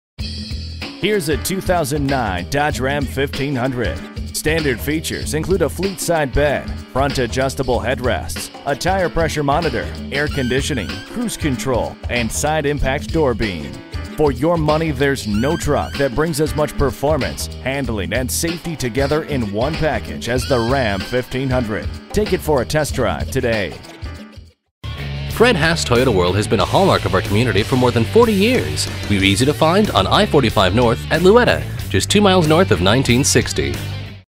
2009 Dodge Ram 1500 Katy Sugarland, TX 9S765516T - SOLD
Year: 2009
Make: Dodge
Model: Ram 1500
Trim: Truck
Engine: 3.7l v6 magnum
Transmission: 4-Speed Vlp Automatic
Color: Inferno Red Crystal Pearl
Interior: Dark slate gray
Mileage: 84245
Stock #: 9S765516T

Address:
20400 I-45 North